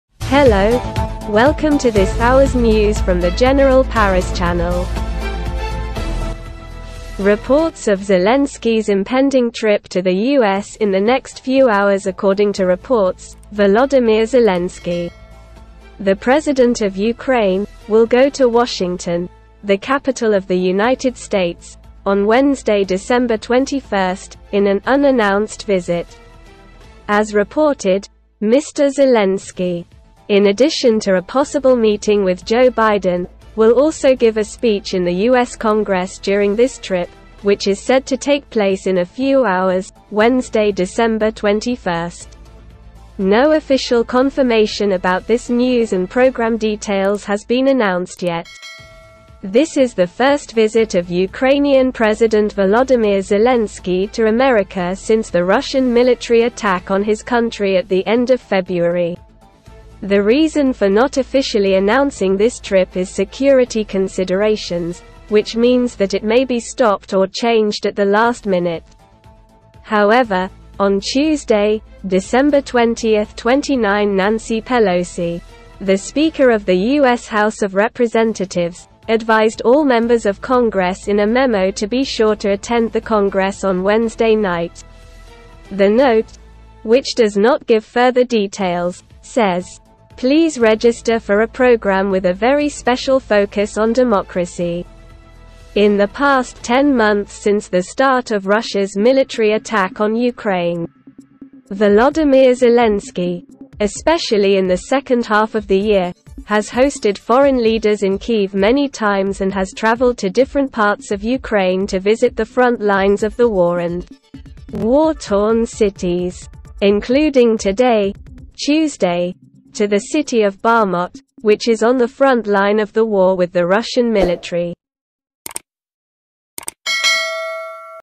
Zelensky's trip to America
According to reports, Volodymyr Zelensky, the president of Ukraine, will go to Washington - the capital of the United States - on Wednesday, December 21, in an unannounced visit. !

General Press
world news
world cup

Thanks for visiting General Press👍